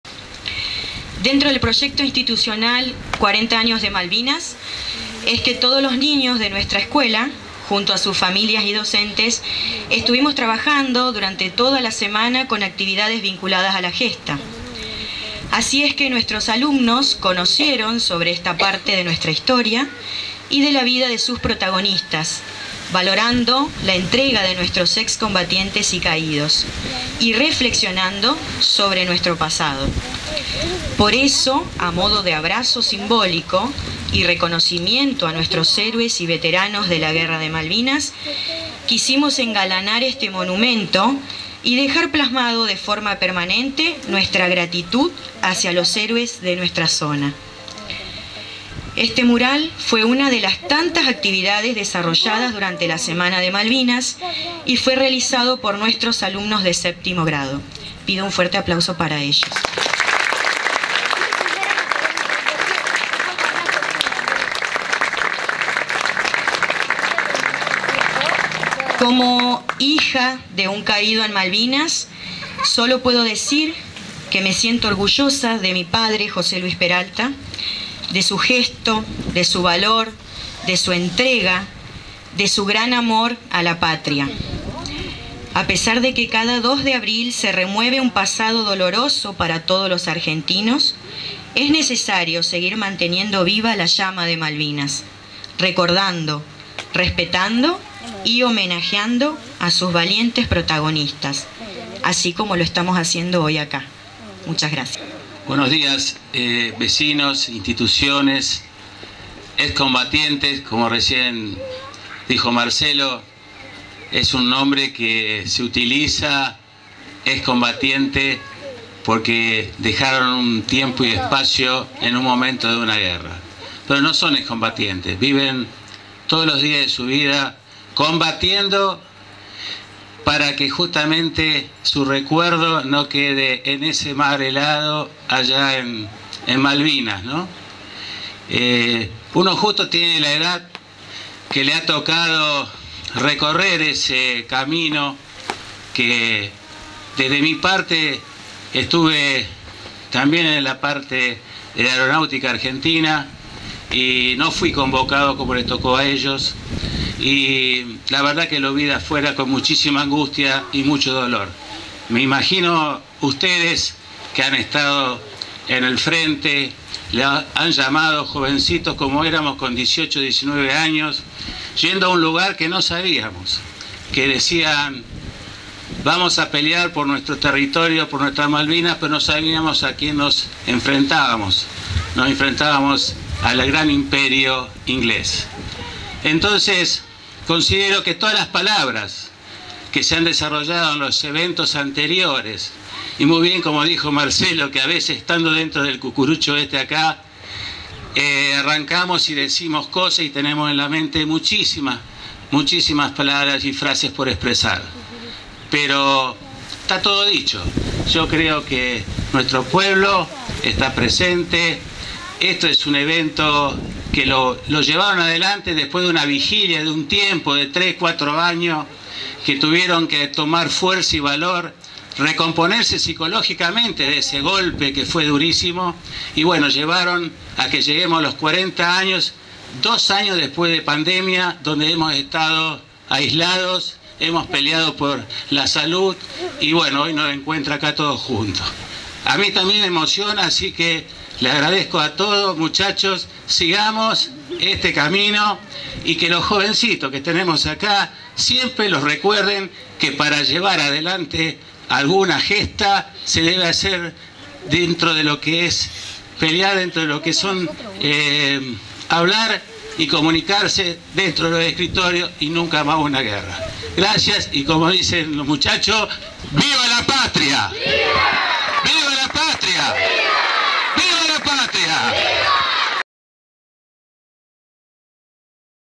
SAN CARLOS NORTE - SE REALIZÓ EL ACTO POR EL 40° ANIVERSARIO DE LA GESTA DE MALVINAS
FUE EN LA PLAZOLETA “SAN CAYETANO”